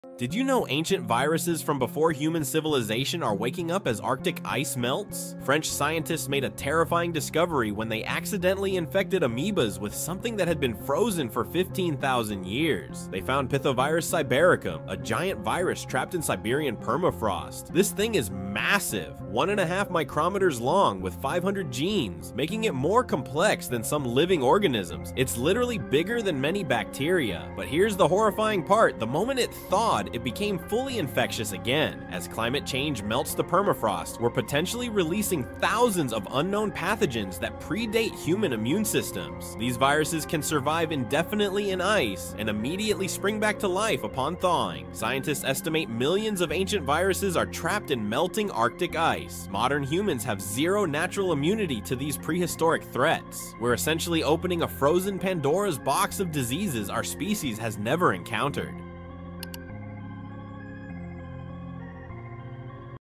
Frozen virus awakes as ice melts ❄️🦠
Glaciers thaw. A long-dormant ancient virus reactivates after ~15,000 years. Labs have already revived permafrost giant viruses (in amoebas), proving survival is possible. What happens if meltwater carries pathogens into wildlife, water systems, or farms and how do genomics & biosafety stop spillover?
👁️ Daily 60-second facts & mysteries — follow for more